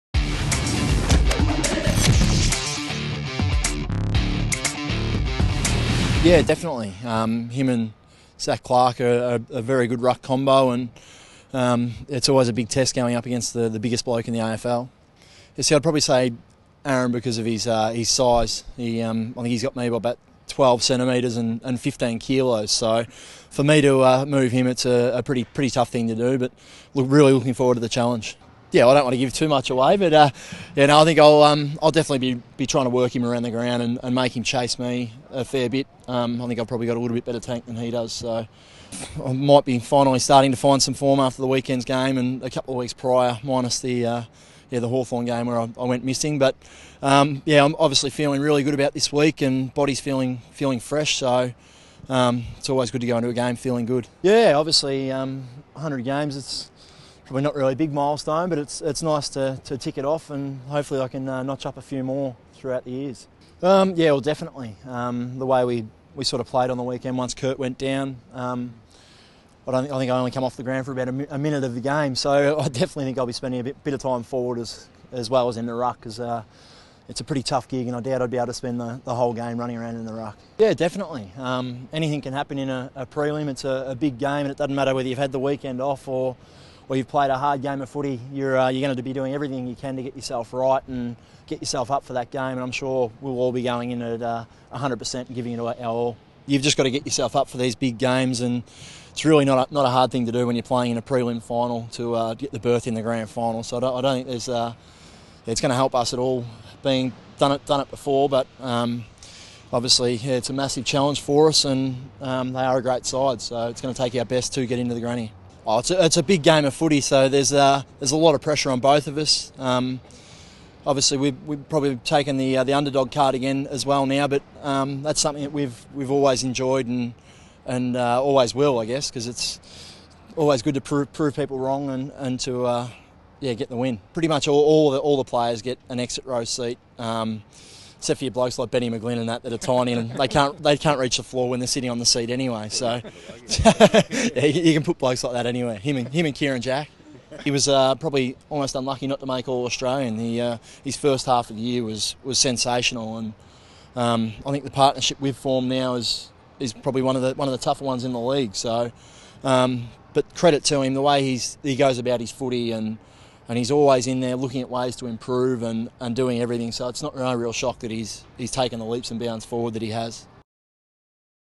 Shane Mumford press conference: Preliminary final week 2013
Swans ruckman Shane Mumford speaks to the media ahead of the sides preliminary final against Fremantle at Patersons Stadium